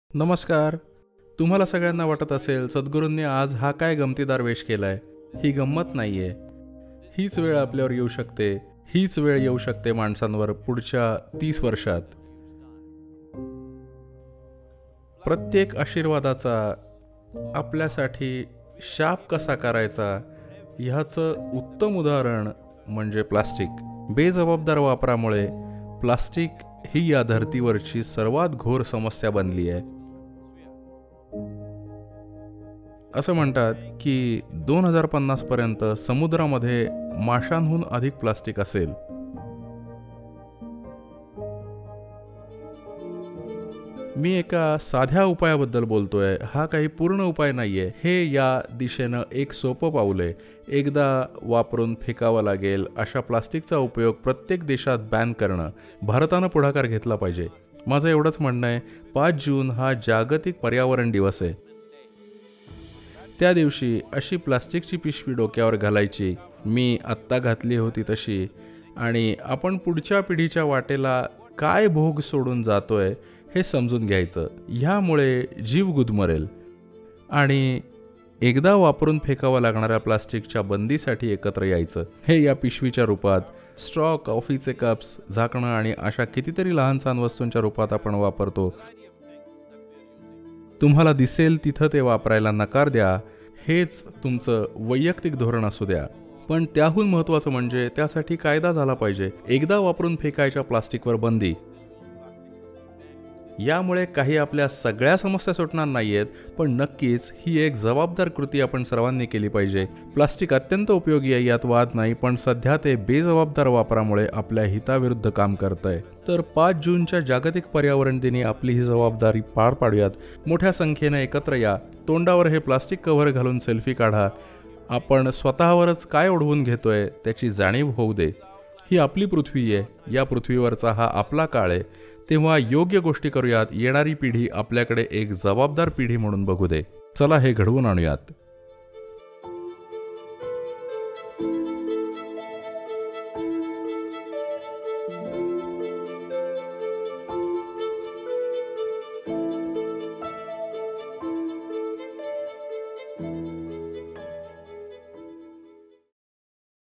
एकेरी वापराचं प्लास्टिक टाळा! सद्गुरुंचा संदेश Ban Single use Plastic World Environment Day 2018
English Video - Ban Single-use Plastic: Sadhguru on World Environment Day 2018

५ जून – आंतराष्ट्रीय पर्यावरण दिन!  सद्गुरुंचा संदेश
• दर वर्षी ८० लाख टन प्लास्टिक समुद्रात टाकलं जातं
• 2050 पर्यंत समुद्रात माशांहून अधिक प्लास्टिक असेल
• दर वर्षी 5000 कोटी प्लास्टिकच्या पिशव्या आपण वापरतो.
• प्रत्येक मिनिटाला 10 लाख प्लास्टिकच्या बाटल्या आपण विकत घेतो.
• आपण निर्माण केलेल्या कचऱ्यात, 10% प्लास्टिक असतं
• एक सोपा उपाय
• सिंगल युज प्लास्टिक – एकदा वापरून टाकण्यजोग्या सर्व प्लास्टिकच्या वस्तू